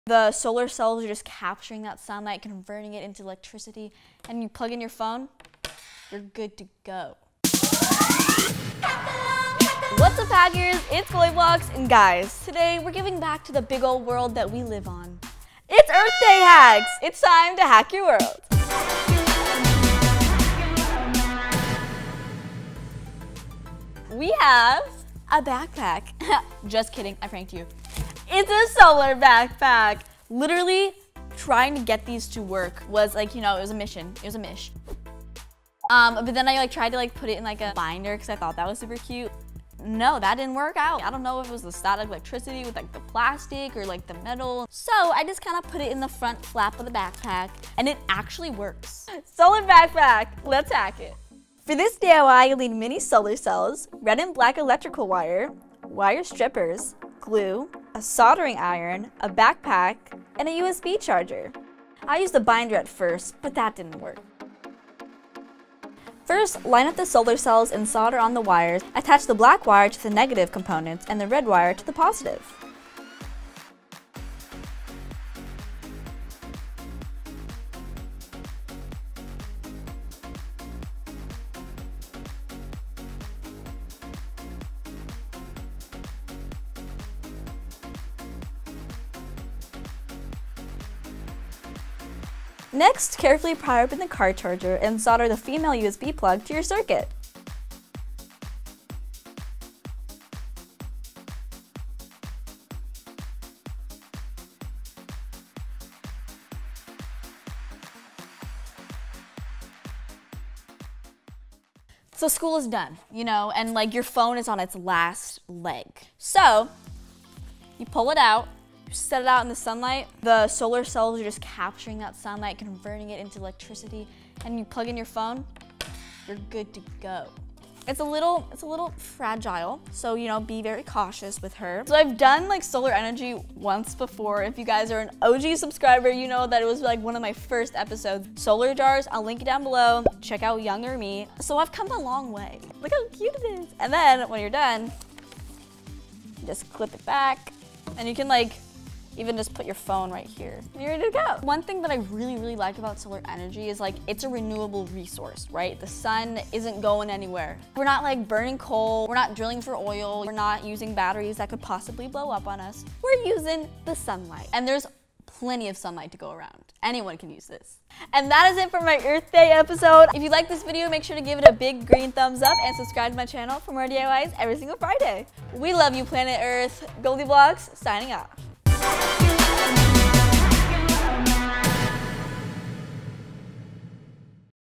STEM Activities for Kids | DIY Solar Backpack Phone Charger! GoldieBlox
Celebrating Earth Day by building a DIY Solar Powered Phone Charging Backpack! Using the power of the sun to keep your phone charged is not only super handy, it also helps the planet!